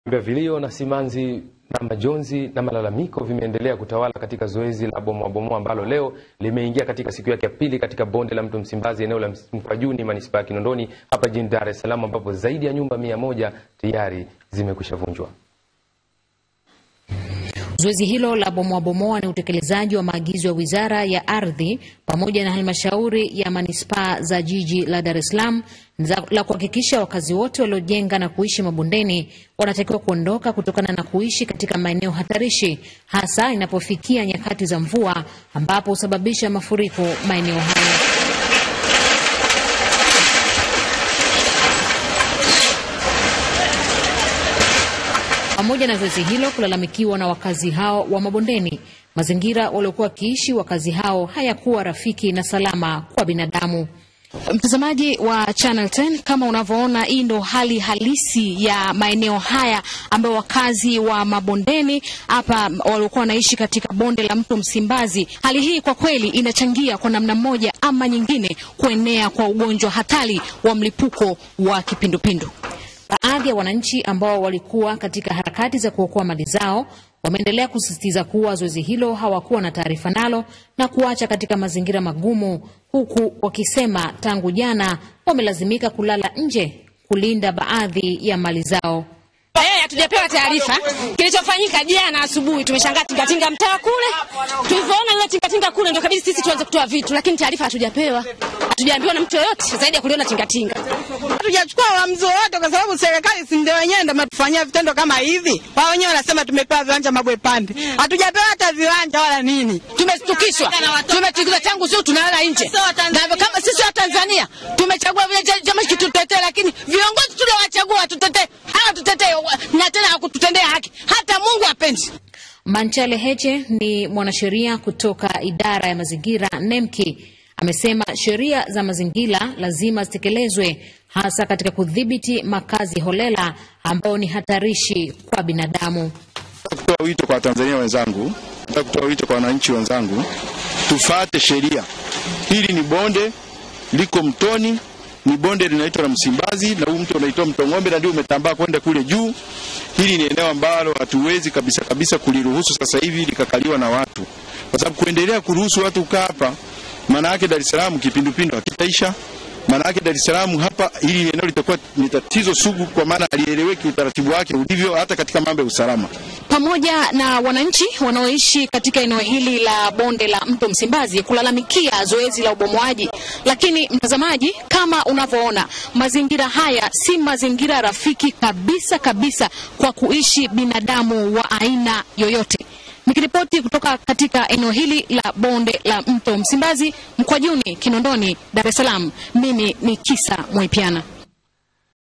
BomoaBomoa Yaendelea Kuliza Wengi Kinondoni
Vilio ,simanzi na majonzi vyaendelea kufuatia zoezi la bomoabomoa kuingia siku ya pili katika eneo la mto msimbazi wilayani Kinondoni.